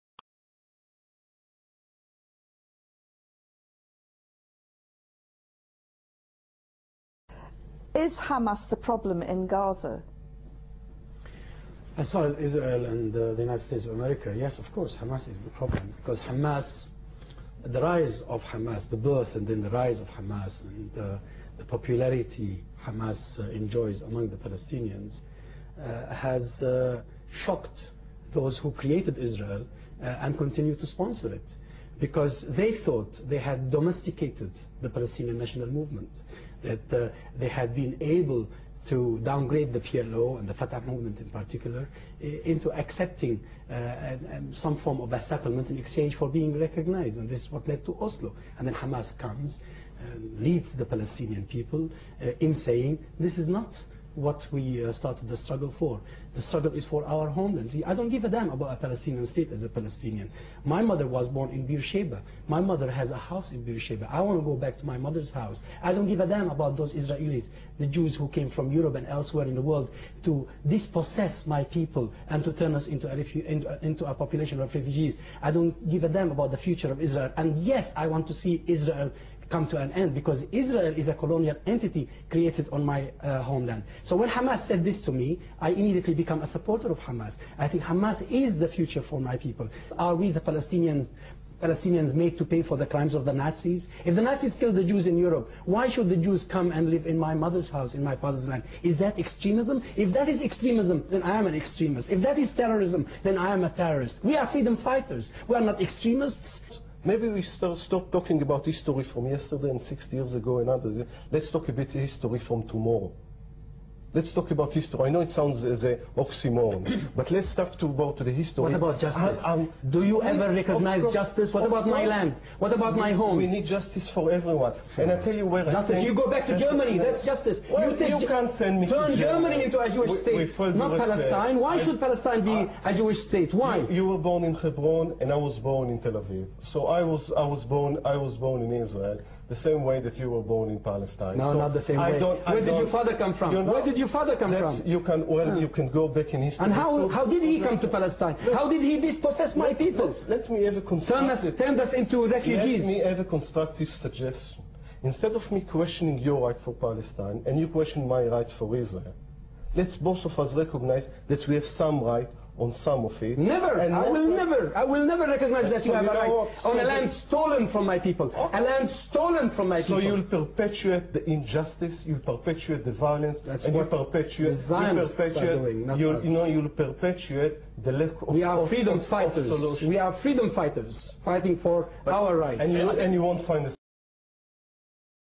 Palestinian Gives a Strong Message to Zionists
The Palestinians won't ever accept occupation of their land and will fight till every part is liberated from the blood thirsty Zionist Regime.